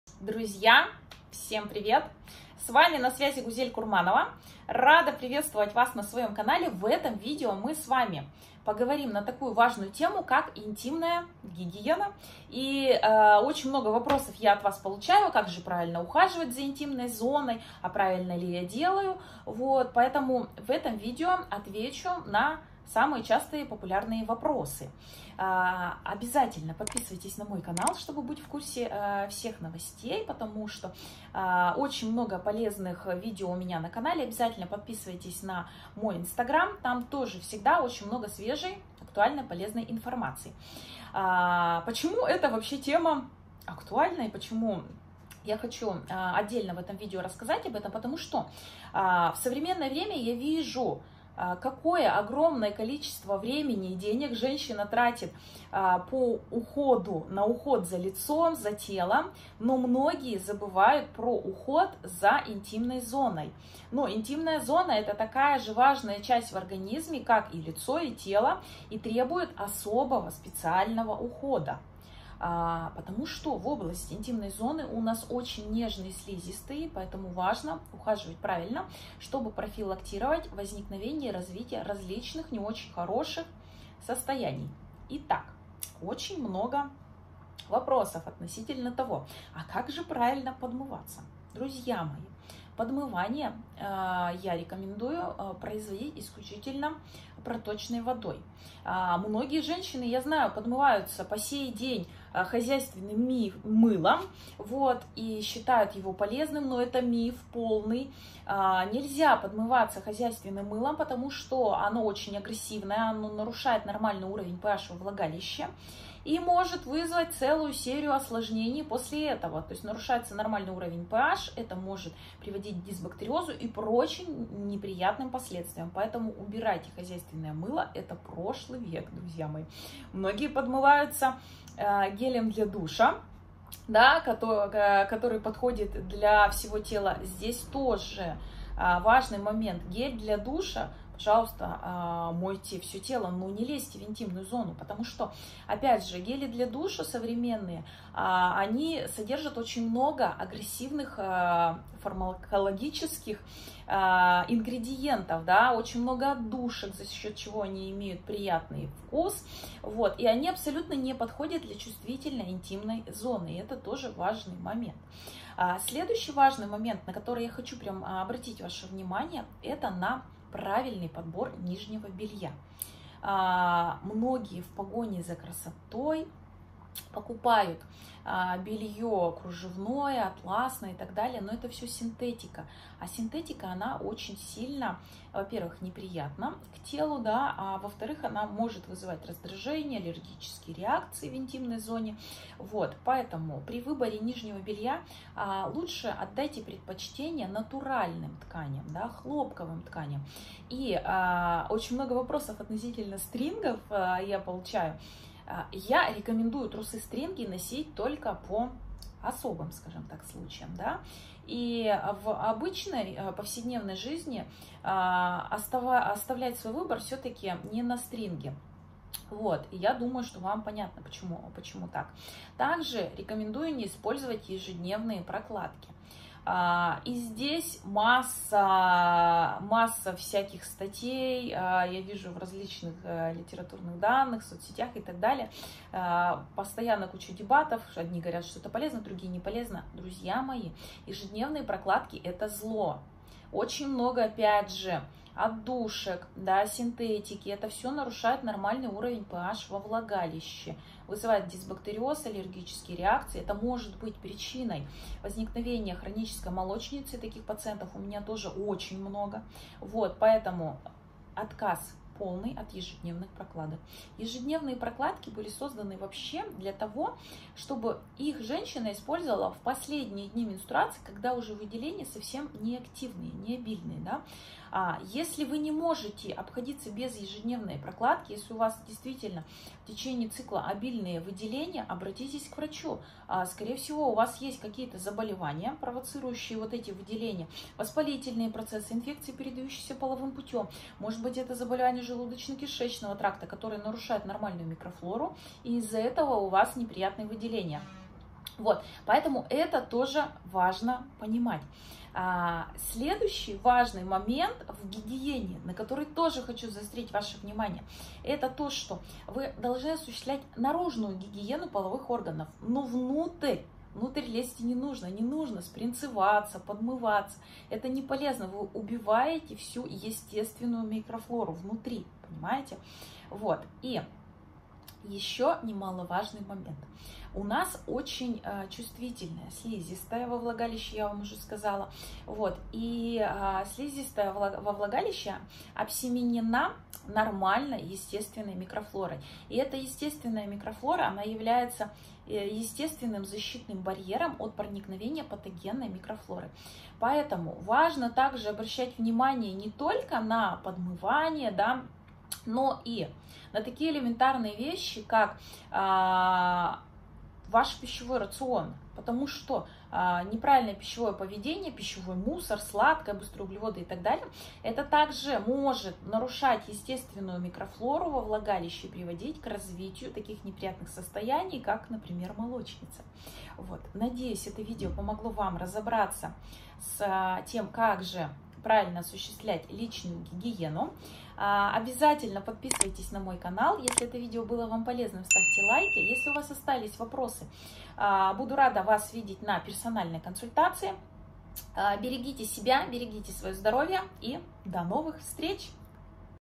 Правила интимной гигиены. Какими средствами вредно пользоваться? Откуда воспалительные процессы?
Тема “Интимная гигиена” весьма щепетильная. Многие по-прежнему используют хозяйственное мыло, которое очень агрессивно. В интимной зоне слизистая очень чувствительная и восприимчивая к различным химическим веществам, отдушках, синтетике и др. Уход за интимной зоной предполагает подмывание проточной водой, ношение белья из натуральных материалов, использование ежедневок иногда и др.

Хозяйственное мыло может нарушить нормальный уровень ph влагалища и у вас появиться дисбактериоз. Аналогичная ситуация может произойти и при использовании гелей для душа. Регулярное ношение стринг может привести к молочнице. Дорогие девочки, стринги, кружевное и атласное белье лучше оставить для “особых” случаев. Не стоит такое белье носить ежедневно. То же самое касается ежедневных прокладок. Есть разные мнения. Изначально они были созданы для последних дней месячных, когда есть еще небольшие выделения. Если в межменструальный период отмечаете обильные выделения, то срочно к врачу на обследование.

🕓 ТАЙМКОДЫ 🕓
0:00 насущные вопросы
1:26 правильное подмывание
2:46 нижнее белье
3:40 вред ежедневных прокладок
5:04 наружный уход
5:55 пищевое поведение